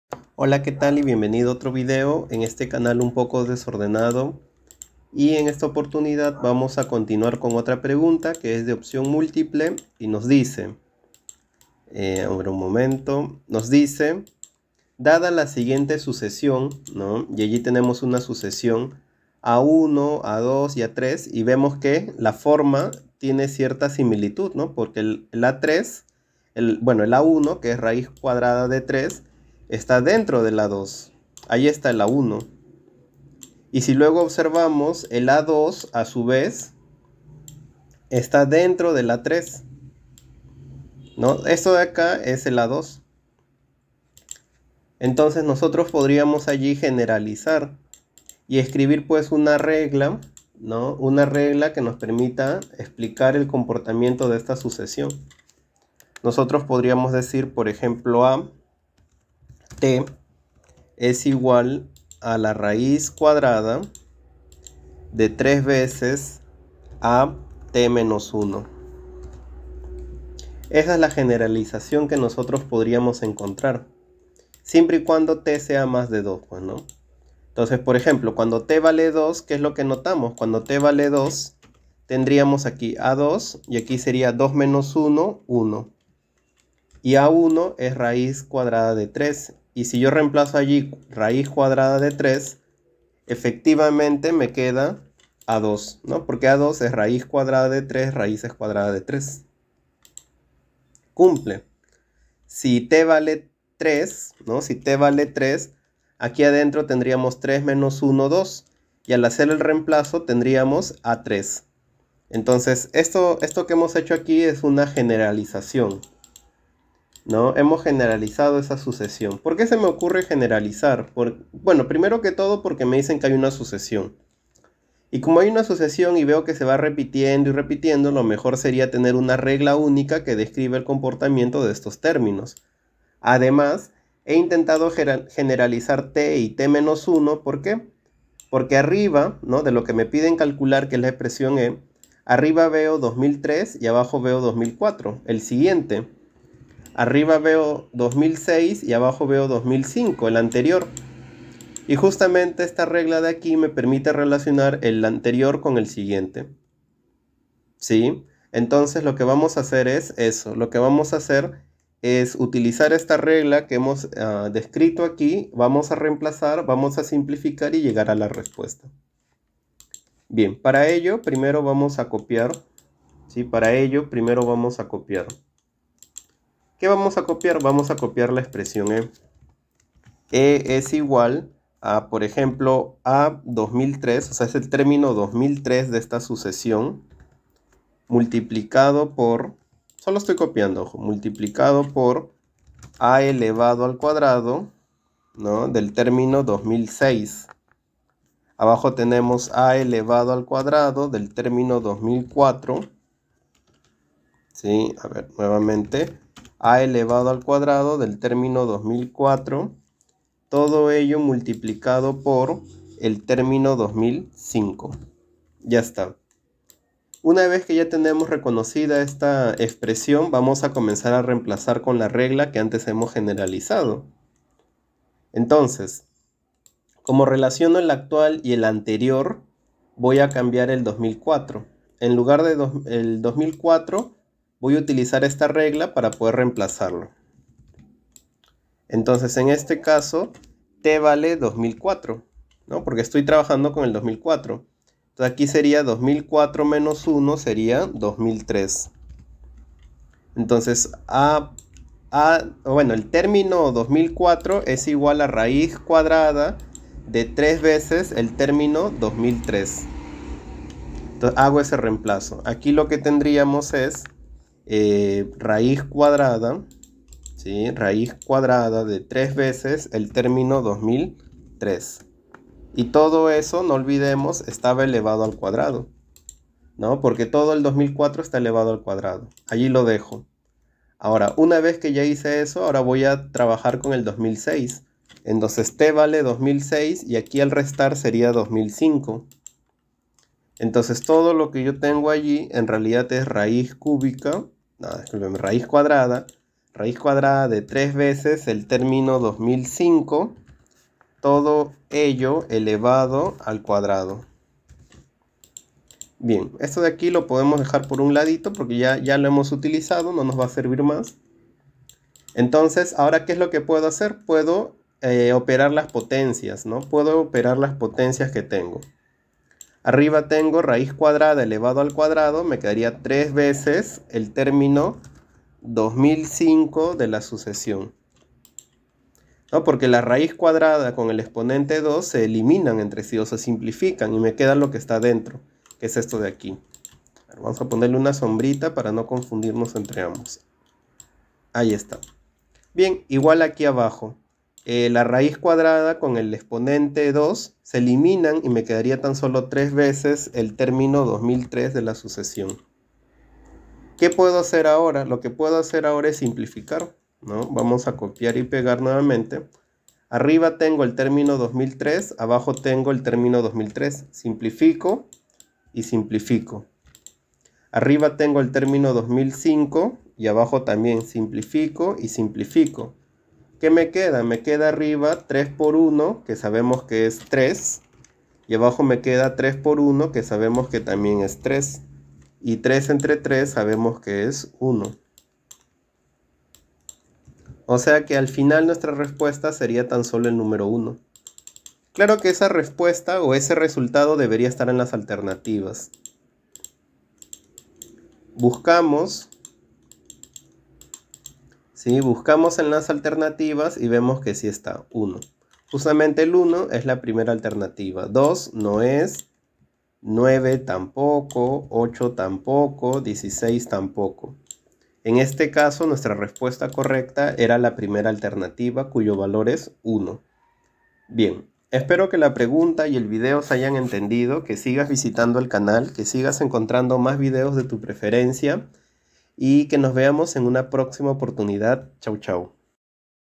1987 - Ejercicio resuelto sobre leyes de exponentes, raíces y fracciones
OPCIÓN MÚLTIPLE
Dada la siguiente sucesión:
a_1=√3, a_2=√(3 √3) , a_3=√(3 √(3 √3) )  …
Calcular:
E=(a_2003×a_2006^2)/(a_2004^2×a_2005 )